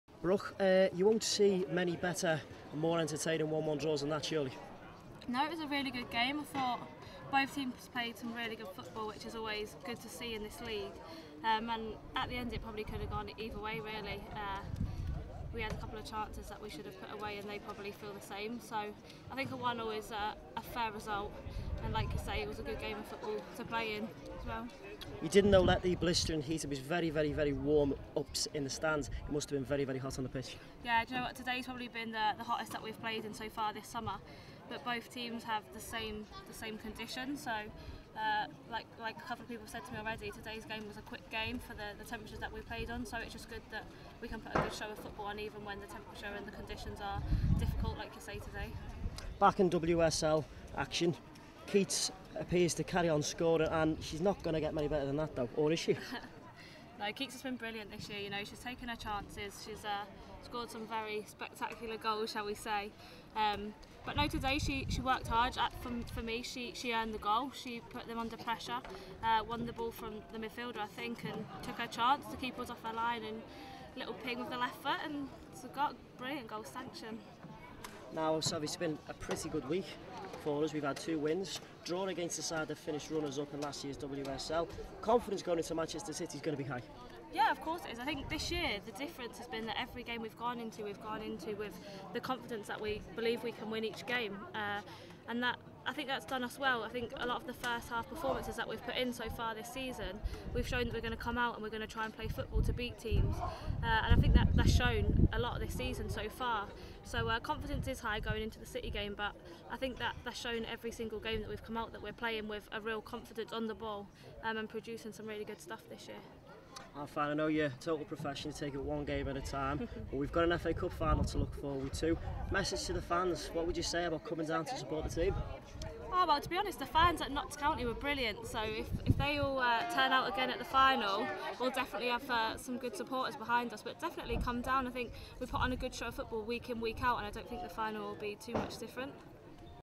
EVERTON LADIES: Brooke Chaplen Post-Bristol
The Everton Ladies midfielder discusses the fantastic performance against Bristol Academy.